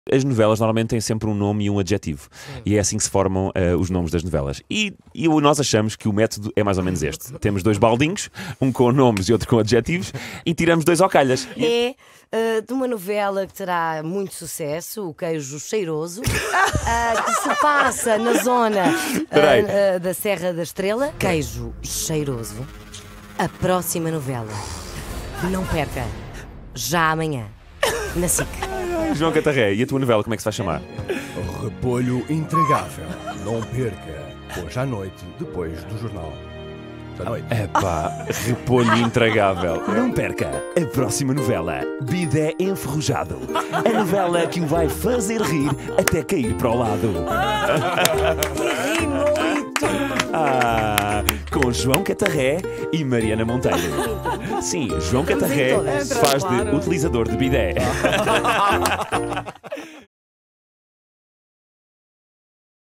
RFM - MARIANA MONTEIRO E JOÃO CATARRÉ - TERRA BRAVA
Desafiámos os atores da nova novela Terra Brava a criar outros nomes para as próximas novelas. Qual foi o que mais te convenceu?